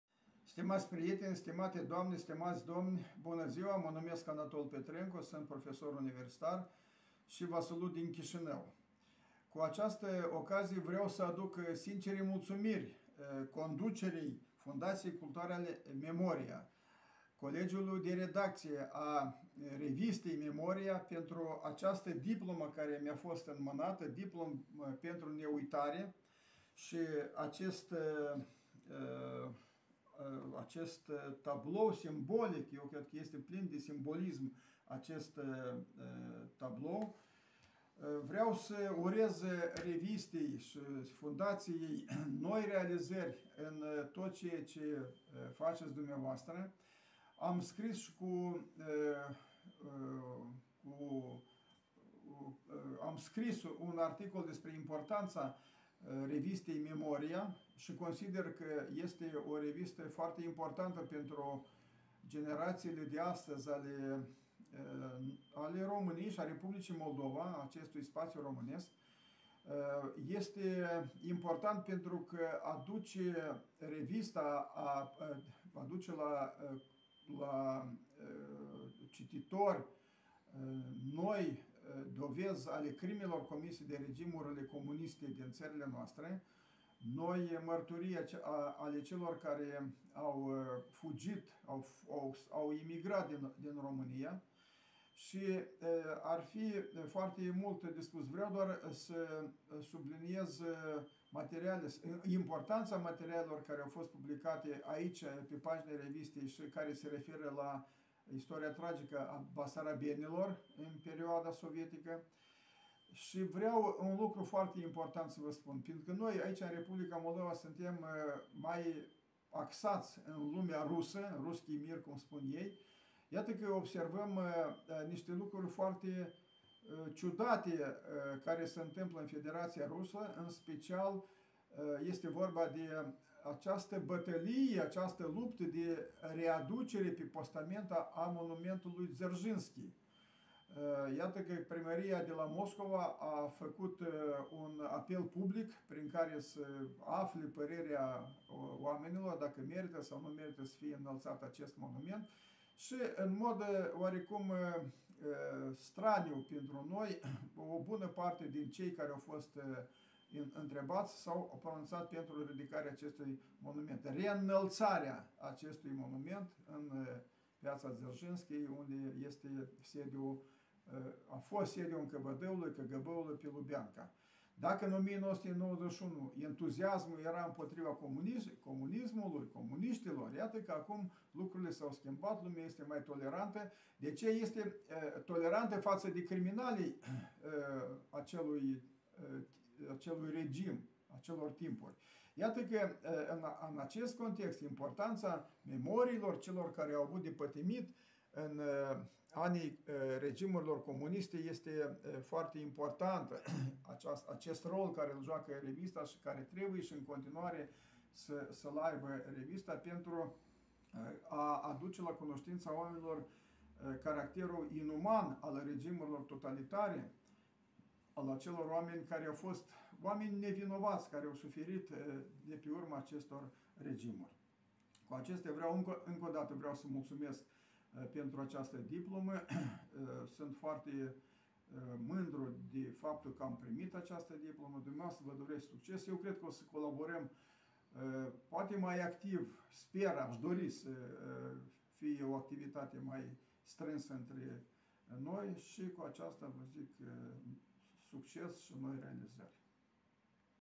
anatol petrencu Institutul de Istorie Sociala ProMemoria
Prof. univ. dr. habil. Anatol Petrencu - istoric, directorul Institutului de Istorie Socială „ProMemoria”, Chișinău, Republica Moldova.
În toamna anului 1990 a apărut primul număr al Memoria - revista gândirii arestate.
La 9 martie 2020, cu ocazia Zilei Deținuților Politici Anticomuniști din Perioada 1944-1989 în România Fundația Culturală Memoria a dorit să organizeze un eveniment aniversar („Gala Memoria - 30 de ani”) prilejuit de cele trei decenii care au trecut de la apariția primului număr al revistei.
În cadrul evenimentului urmau să se acorde o serie de distincții unor personalități ale societății civile românești sau instituții implicate în ultimii 30 de ani în prezervarea și valorificarea memoriei trecutului recent.
Din cauza pandemiei de Covid 19 evenimentul a fost anulat și am fost în imposibilitatea de a-l reorganiza. Cele 11 distincții însoțite de „diplome de neuitare” au fost înmânate ulterior în afara unui cadru festiv.
Redăm în continuare mulțumirile celor care au primit aceste modeste „semne” ale recunoștinței noastre, în ordinea în care ar fi trebuit să le rostească de pe scena Ateneului Român în 9 martie 2020,